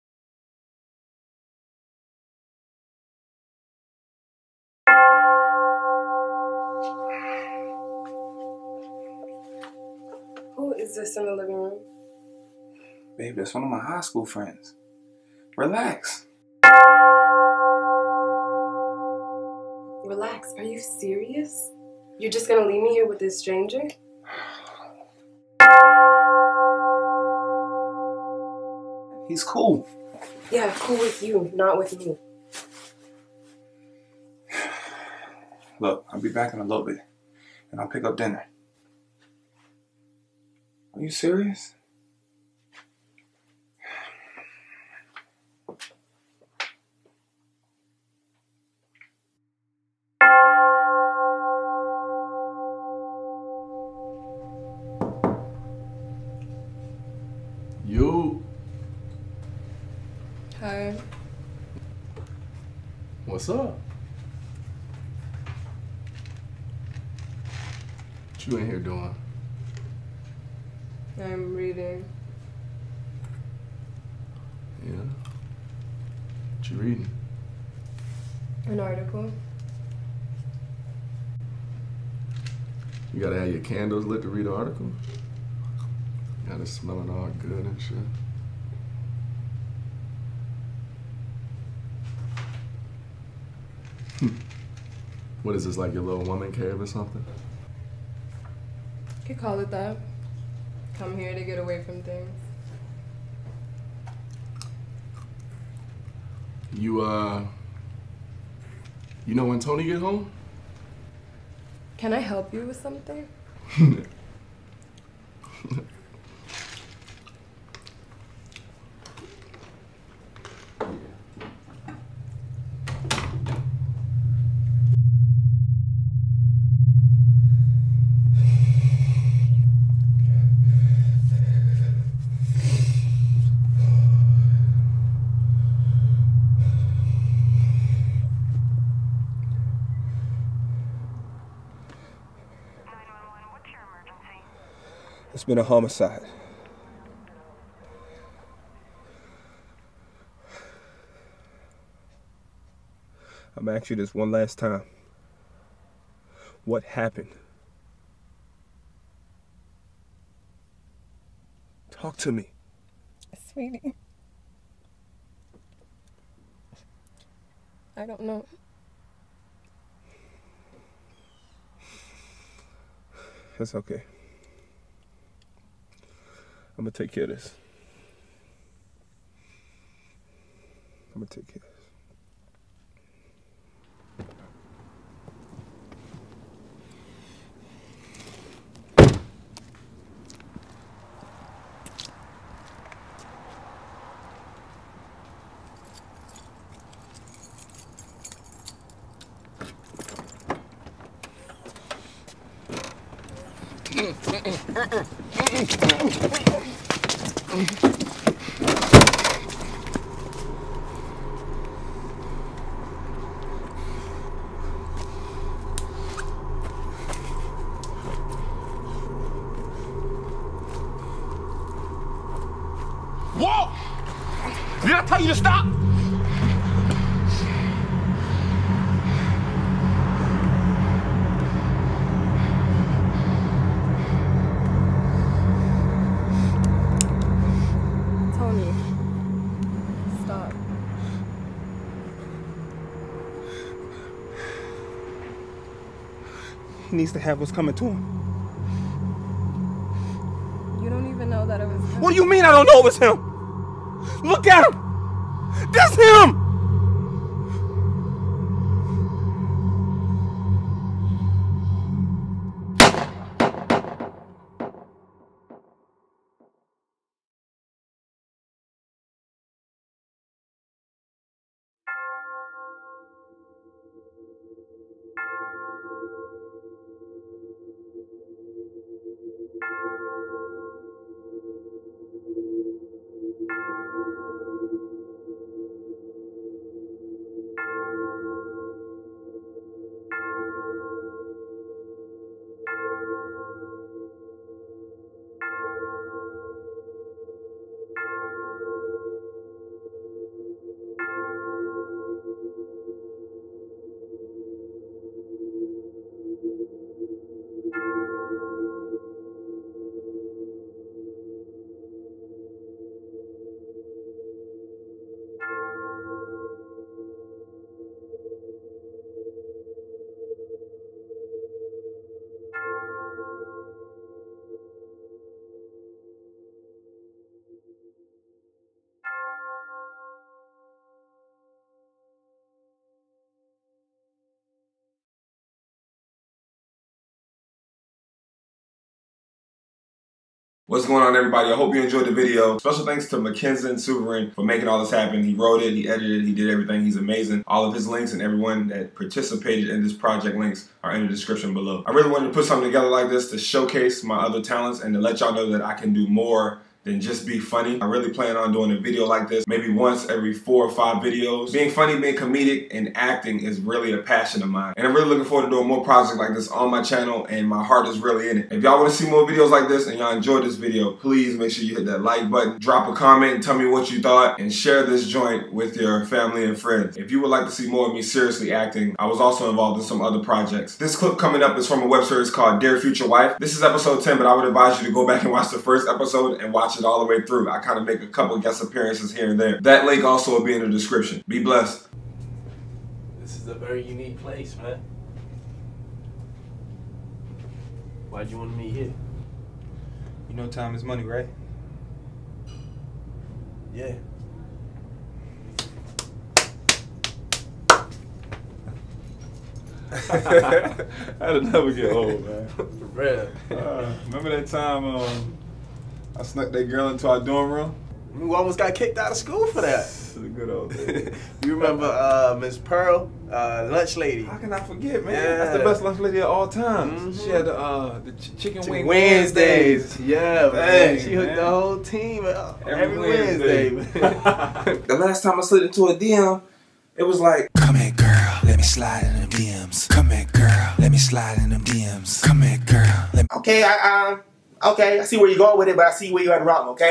UNEXPECTED
Starring:
Tpindell - @Tpindell
Morgan Morgan - @Cordial.Venom
Rich Morrow - @Just_Richboy

Written by:
McKinson Souverain: @McKinSkin

Audio by:
Bieness Alfred: @Bee4orreal

The Unexpected happens when roommate violates his girl.